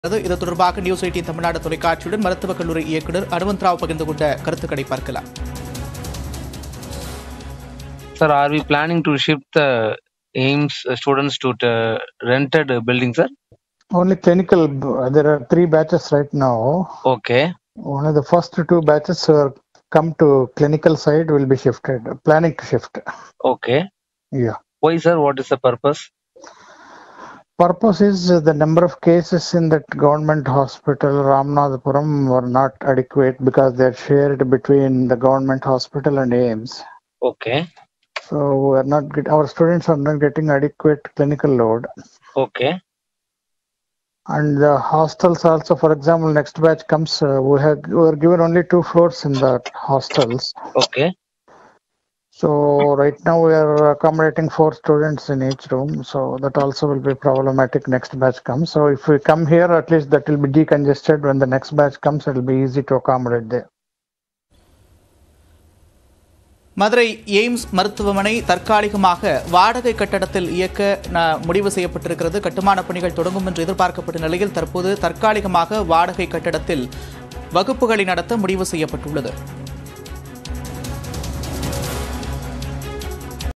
வாடகை கட்டடத்திற்கு மாற்றப்படும் மதுரை எய்ம்ஸ் மாணவர்கள் - காரணத்தை விளக்கிய எய்ம்ஸ் இயக்குநர்
வாடகை கட்டடத்திற்கு மாற்றப்படும் மதுரை எய்ம்ஸ் மாணவர்கள் - காரணத்தை விளக்கிய எய்ம்ஸ் இயக்குநர் அனுமந்த்ராவ்.செய்திகளை துல்லியமாகவும் துரிதமாகவும் வழங்கும் தமிழகத்தின் முதன்மை செய்தி தொலைக்காட்சி உங்கள் நியூஸ்18 தமிழ்நாடு.